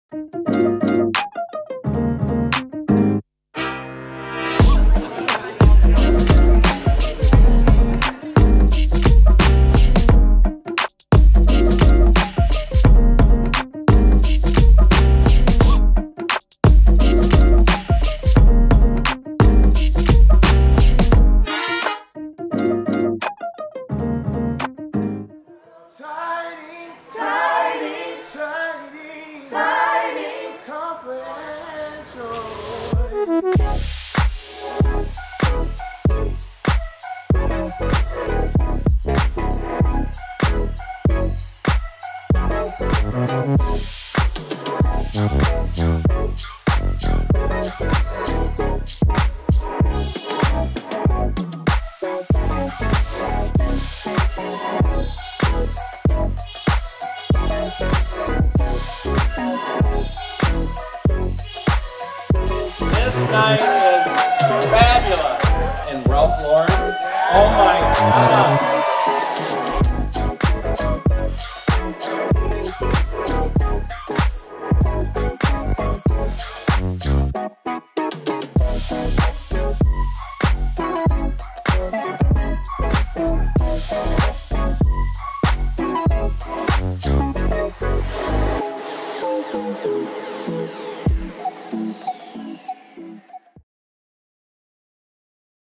The Holidays with Ralph Lauren: An A.K. Rikk's Event Experience
We are still basking in the holiday glow from our black tie event with Ralph Lauren! Our clients are an extension of our family, and we truly felt like we were at a big, beautiful family reunion as we ate, drank, and danced the night away with each other. What a wonderful excuse to come together and celebrate such an iconic brand and the season ahead!
From the bottom of our hearts, we are so grateful to everyone in attendance and also our very special vendors who helped create a night to remember!

⁠Want to get invited to the next event? Make sure you sign up for our emails and reach out to your Personal Shopper for more details!